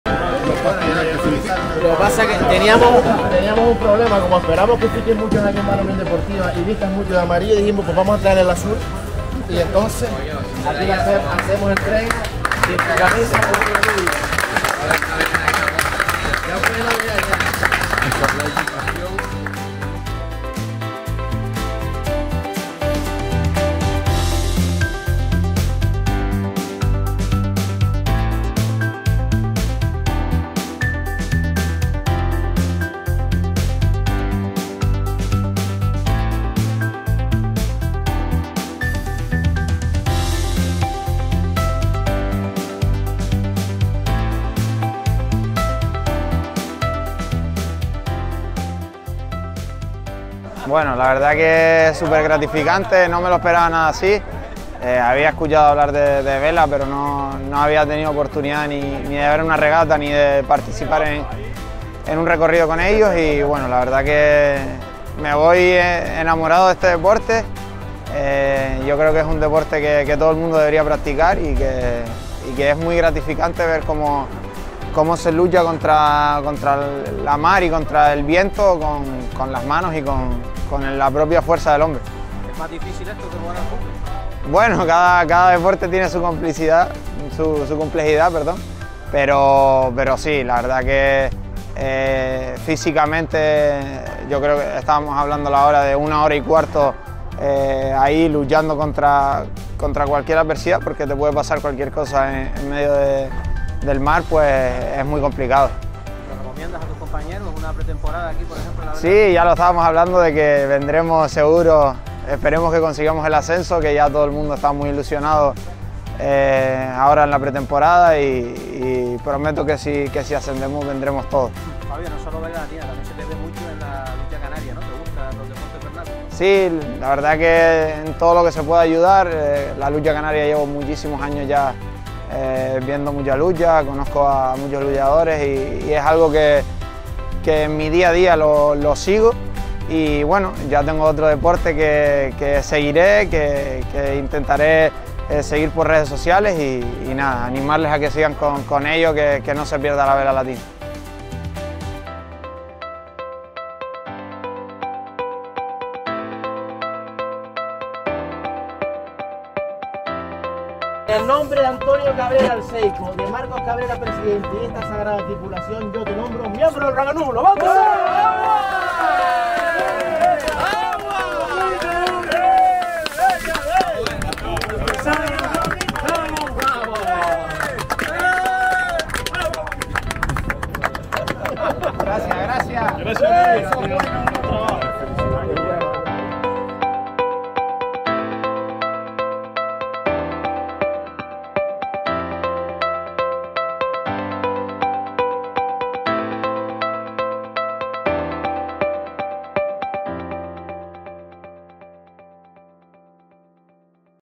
Fabio González se bautiza en la VelaLatinaCanaria
El jugador de la UD Las Palmas, Fabio González vivió una jornada diferente en sus últimos días de vacaciones antes de iniciar la pretemporada con el primer equipo amarillo. El mediocentro, que recientemente ha renovado su contrato por tres temporadas se bautizó en la Vela Latina Canaria, salió encantado con la experiencia y con una promesa.

“Si ascendemos me traigo a todo el equipo”, esa promesa lanzó Fabio González a los tripulantes del bote DISA Roque Nublo ULPGC nada más llegar al Muelle Deportivo tras ser uno más en la embarcación y recibir el bautizo de mar de manos de Luis Azofra.

El de Ingenio es un amante de los deportes vernáculos, conocida es su pasión por la lucha canaria y rara es la semana que no se le ve en algún terrero de la isla siguiente alguna luchada. A esta pasión se le unirá ahora la Vela Latina Canaria. "Me voy enamorado de este deporte, todo el mundo debería practicarlo. Es muy gratificante ver cómo se lucha contra el mar y contra el viento sólo con las manos y la fuerza del hombre".

Fabio no dudó en destacar la complejidad de la práctica de la Vela Latina Canaria. “Físicamente estar una hora y cuarto luchando contra cualquier adversidad es muy complicado”, señaló tras ser uno más de la tripulación del DISA Roque ULPGC.

El mediocentro amarillo se mostró ilusionado con la nueva temporada de la UD Las Palmas: “Esperemos que consigamos el ascenso, estamos todos muy ilusionados y si lo conseguimos vendremos todos a practicar Vela Latina Canaria”, sentenció.

Fabio González hizo banda como uno más, fue bautizado y quiso devolver la atención del bote Disa Roque Nublo ULPGC entregándoles una camiseta de la UD Las Palmas.